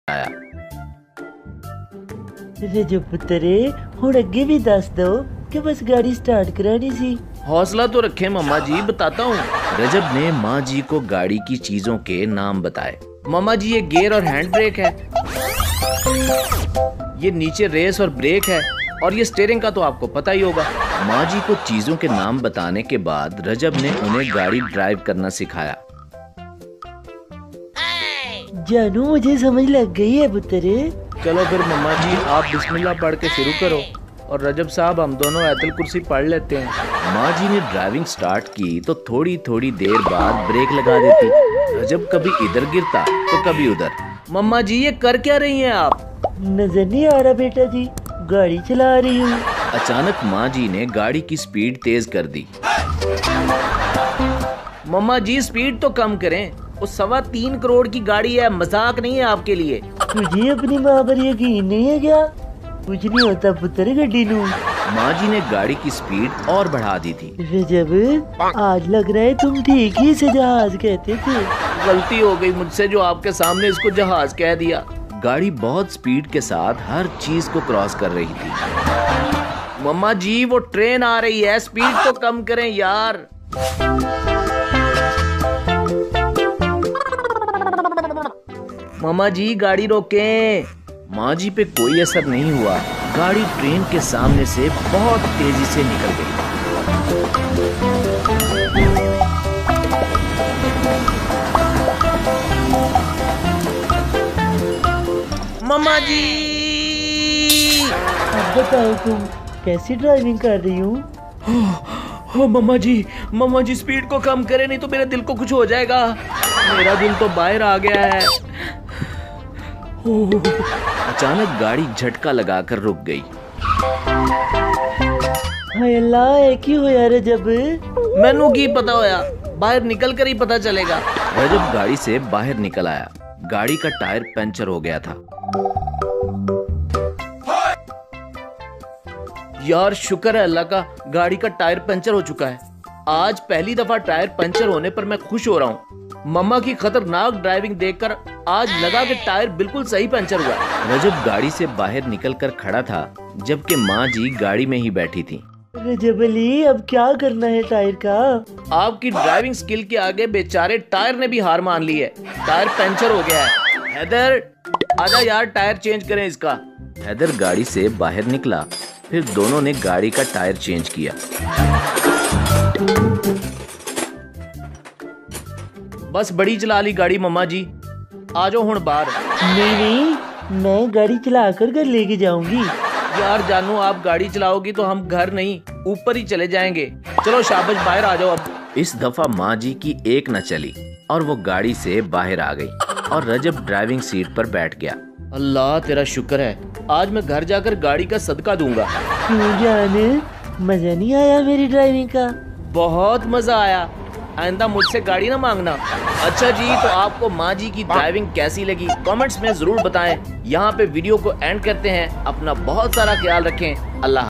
Rajab Butt ny Maa g ko Driving sikha di😋Part 2 |Lahori boy vlogs#
Lahori boy vlogs#